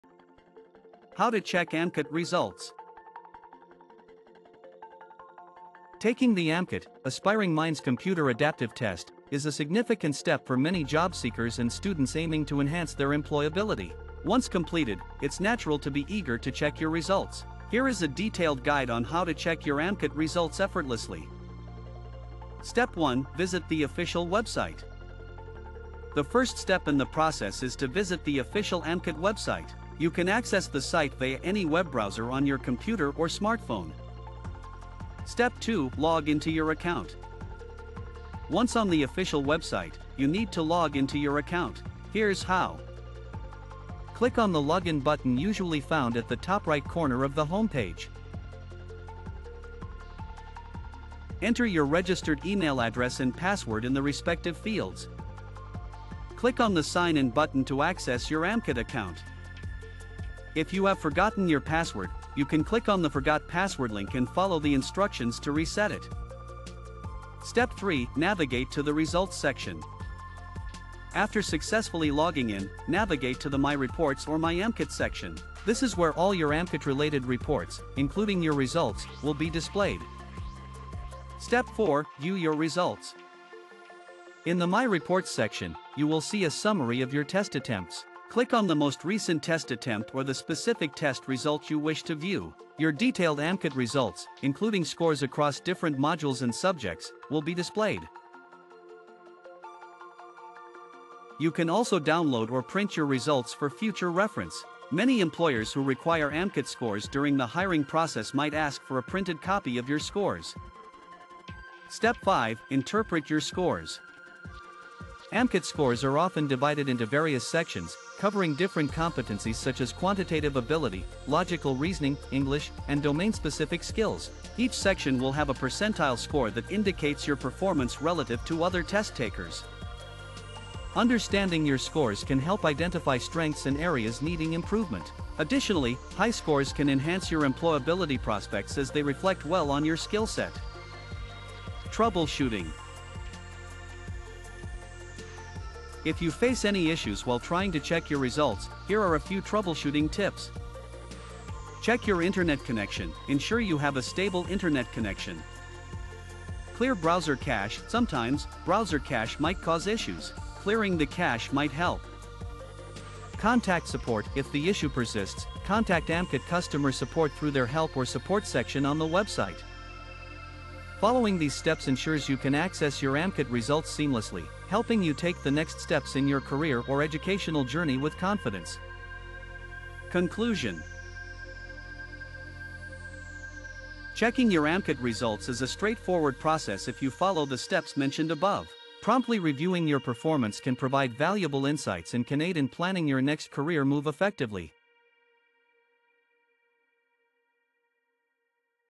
How to Check AMCAT Results
Learn how to easily check your `AMCAT` results with this comprehensive step-by-step guide.
How to Check AMCAT Results: A Step-by-Step Guide

Taking the AMCAT (Aspiring Minds Computer Adaptive Test) is a significant step for many job seekers and students aiming to enhance their employability. Once completed, it's natural to be eager to check your results. Here is a detailed guide on how to check your AMCAT results effortlessly.

The first step in the process is to visit the official AMCAT website. You can access the site via any web browser on your computer or smartphone.

Step 2: Log In to Your Account

Once on the official website, you need to log in to your account. Here’s how:

Click on the “Login” button usually found at the top-right corner of the homepage.

Click on the “Sign In” button to access your AMCAT account.

If you have forgotten your password, you can click on the “Forgot Password” link and follow the instructions to reset it.

Step 3: Navigate to the Results Section

After successfully logging in, navigate to the “My Reports” or “My AMCAT” section. This is where all your AMCAT-related reports, including your results, will be displayed.

Step 4: View Your Results

In the “My Reports” section, you will see a summary of your test attempts. Click on the most recent test attempt or the specific test result you wish to view. Your detailed AMCAT results, including scores across different modules and subjects, will be displayed.

You can also download or print your results for future reference. Many employers who require AMCAT scores during the hiring process might ask for a printed copy of your scores.

Step 5: Interpret Your Scores

AMCAT scores are often divided into various sections, covering different competencies such as Quantitative Ability, Logical Reasoning, English, and Domain-specific skills. Each section will have a percentile score that indicates your performance relative to other test-takers.

Understanding your scores can help identify strengths and areas needing improvement. Additionally, high scores can enhance your employability prospects as they reflect well on your skill set.

Troubleshooting

If you face any issues while trying to check your results, here are a few troubleshooting tips:

Check Your Internet Connection: Ensure you have a stable internet connection.

Clear Browser Cache: Sometimes, browser cache might cause issues. Clearing the cache might help.

Contact Support: If the issue persists, contact AMCAT customer support through their “Help” or “Support” section on the website.

Following these steps ensures you can access your AMCAT results seamlessly, helping you take the next steps in your career or educational journey with confidence.

Conclusion

Checking your AMCAT results is a straightforward process if you follow the steps mentioned above. Promptly reviewing your performance can provide valuable insights and can aid in planning your next career move effectively.